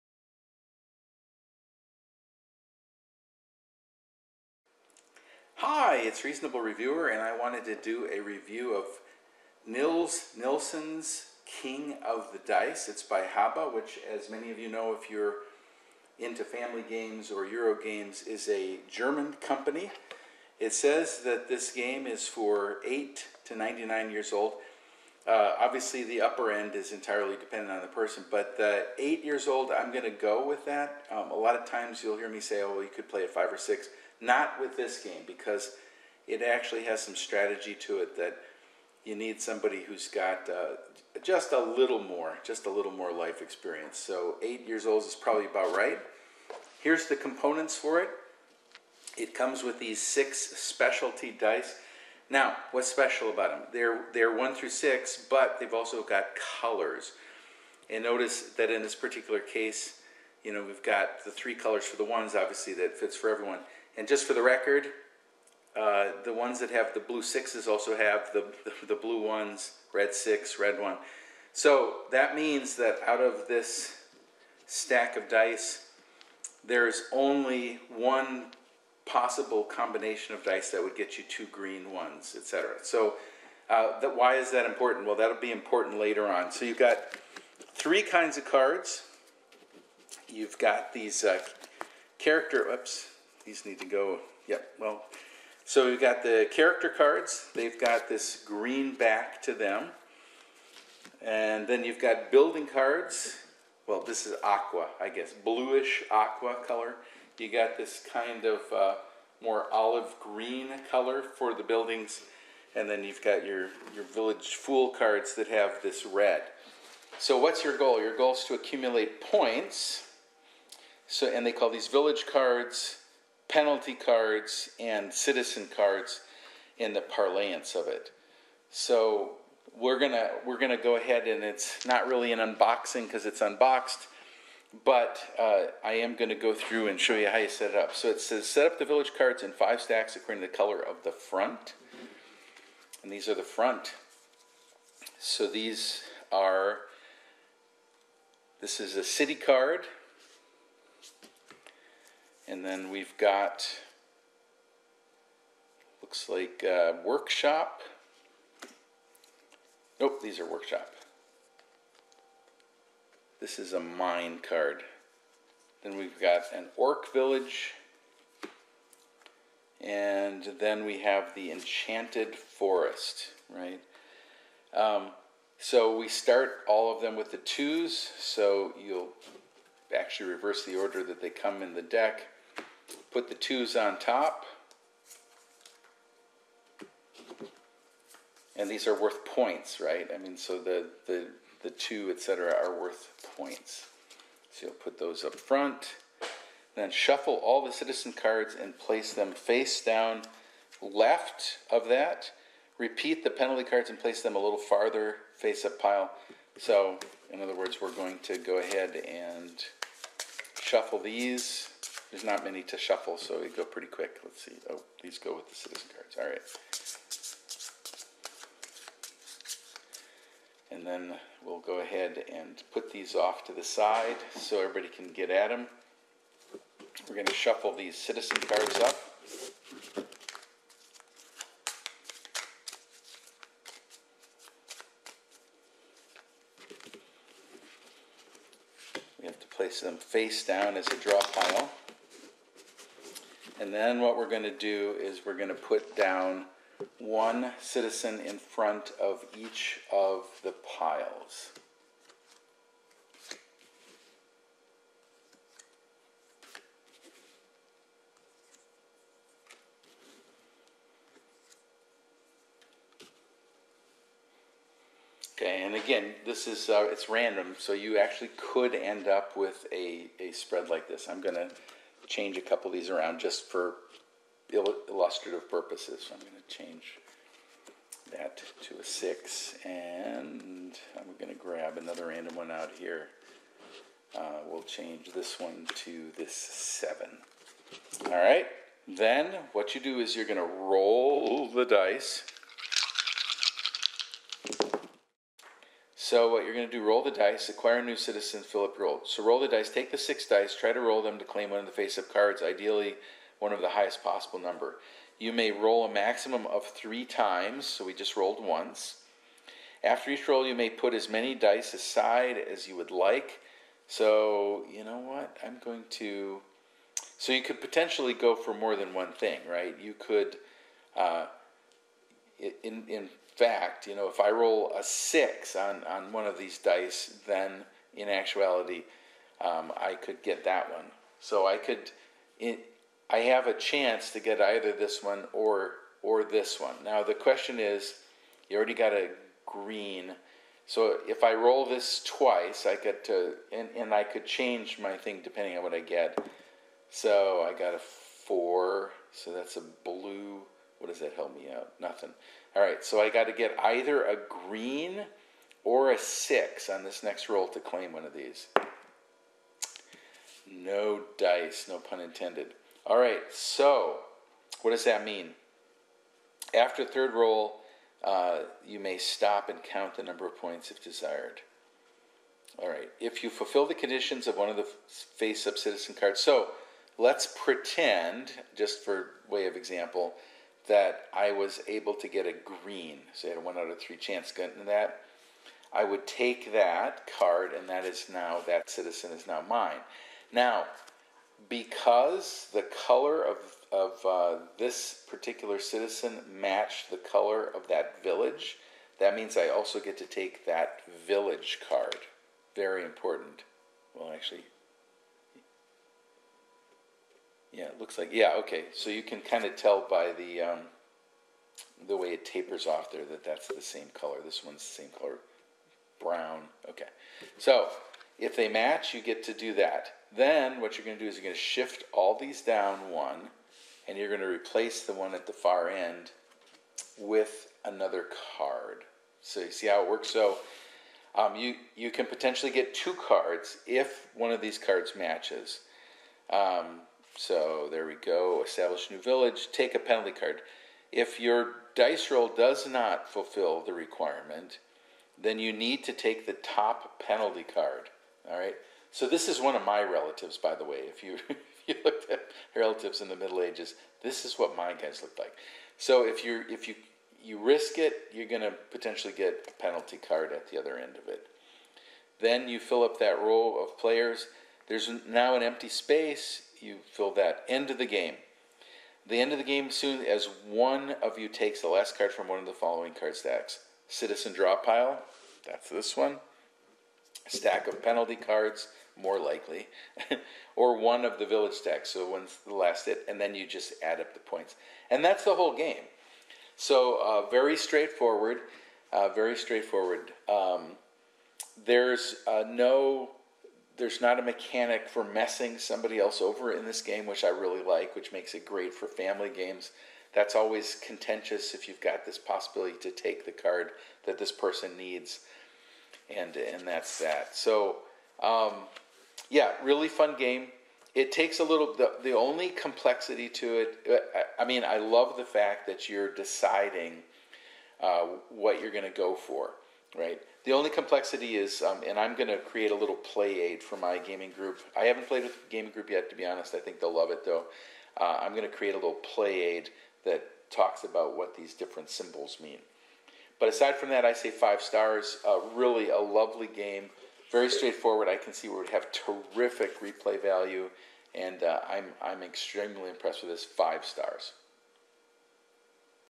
Review King of the Dice
This is a review of the dice game, King of the Dice.

Summary:  King of the Dice is a game for 2-5 players, ages 8+, that will play in about thirty minutes. The game was designed by Nils Nilsson and distributed by Haba games.

There are six specialized, six sided dice. Each of the dice has the numbers one to six, but they are also a particular color, red, green, or blue. There are two dice with the possibility of getting a blue one or a blue six, two dice that can get a red one or a red six, etc.

Sometimes you will be trying to get certain numbers, or odd/even numbers, or totals. Sometimes you will be trying for series (numbers in sequence), and sometimes you will be looking for combinations of colors and some combinations have both numbers and colors specified.

The game comes with three separate types of cards, village cards, penalty cards, and citizen cards.

At the beginning of the game, the village cards are laid out in five stacks, corresponding to the five types of villages: city, mine, workshop, orc village, or enchanted forest.

Below the five village piles, you place one villager. Each of these villagers has a particular combination of colors and numbers that you need to obtain on the dice to claim the villager. You get to roll the dice three times maximum (you can stop if you have a combination you want). For the first roll, you roll all the dice. For the second and third rolls, you select which dice you want to re-roll.

If you end up with a combination of dice results corresponding to a villager, you can claim that card. If the color/village of the villager corresponds to the village card above it then you *also* claim that village card.

If you do not get a combination of dice results that allows you to claim a villager, you have to take one of the penalty cards.

If you did take a villager then you slide the remaining villager cards left of the opening one space right to fill the gap and turn over a card from the draw pile to fill the left-most slot.

In this way, a card might line up under a village of its same color/type.

Each character and village card has a number of points on it, victory points. The one exception if the busy fairy card that has an "X" on it. The X means that every fairy card has a point value equal to the number of fairies you collected. One fairy = 1 x 1 or one points. Two fairies = 2 x 2 or four points, etc.

The game ends when you draw the last: penalty card, citizen from the citizen pile, or the last village card in one of the five stacks.

Players add up their points, and high point total wins. Ties are possible.

On to the Dr Games’ Criteria …

GO/NO GO Criterion

• Complexity: (GO)  The game has some complexity. A kiveat chart on the back of the chart says that this is firmly in the luck prevails realm, but our gaming group found that there was a fair amount of strategy involved with knowing which were the best cards to aim for in your dice rolling. The eight year old suggested minimum age is a good one for this game.

• Balanced: (GO )   The game is utterly balanced. Anyone can win, and everyone will have fun.

• Chance (GO)  There is chance involved in which villager cards come up and how the dice rolls go, but it is very much informed chance, and a winning strategy will prevail most of the time. If you have a choice between several villagers on your first roll, try to keep dice combinations that have a chance of matching multiple villagers. If you "go for broke" and try the "risk all, win all" strategy then you might or might get the points. We found that lower risk choices generally won the day.

• Clarity: (GO)   The rules are completely clear.

• Reasonable Time: (GO)  The rules suggest that this is a 30 minute game, and that seems about right. Your first couple of times playing might go longer, but the 4+ time you play, the game will last about half an hour.

Bonus Criteria

• Social:  The cards are cute, and this leads to some fun discussions especially if some of your more theateric players want to imitate the arrogant elf, grumpy dwarf, or cuddly orc. There is nothing in the rules that is either overtly social or non-social.

• Unique/Interesting Mechanics:  The X factor with the fairy and the ability to capture villages as well as villagers allows for some strategic planning.

• Informed:  Not in this game.

• Rewards Throughout:  Yes, once you capture a card, its points are yours till the end of the game.

All in all, it is a fun, well designed game for the entire family.